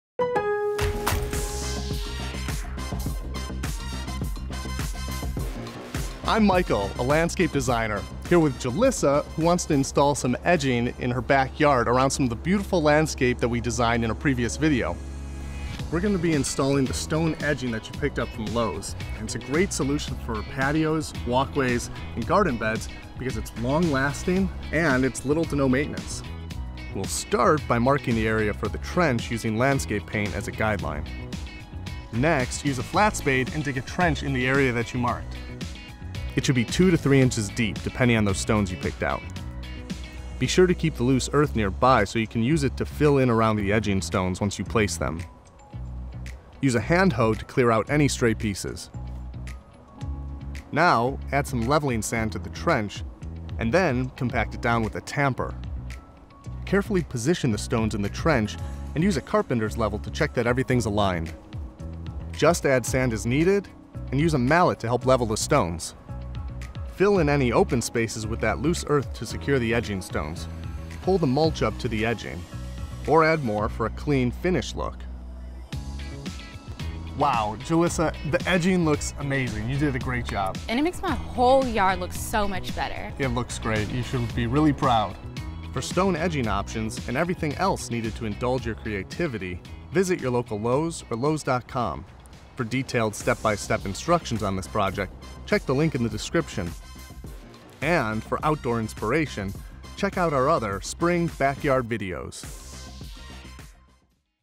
How To Install Landscape Edging
In this video you will learn how to install landscape edging so you can add definition and beauty to any landscaping project. It’s a great solution for outlining patios, walkways, and gardens since it’s long-lasting and requires little to no maintenance.